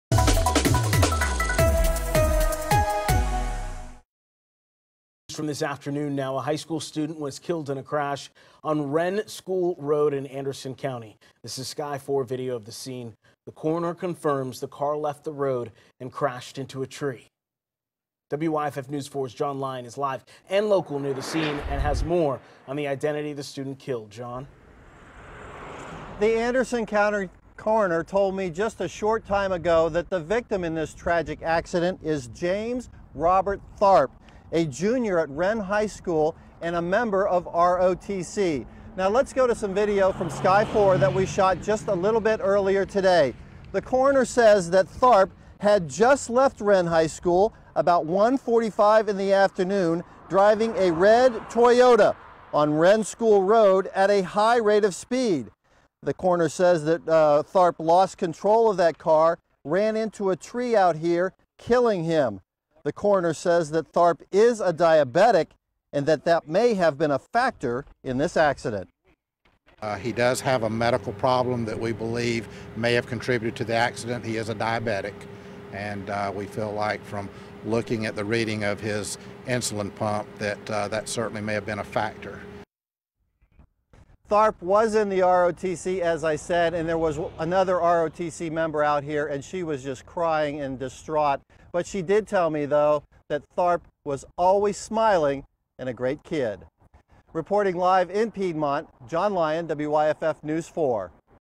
17 year old high school student killed in crash in Anderson County
The Anderson County Coroner says the victim, James Robert Tharp, was a Wren High School student and may have went into a diabetic shock before the crash Subscribe to WYFF on YouTube now for more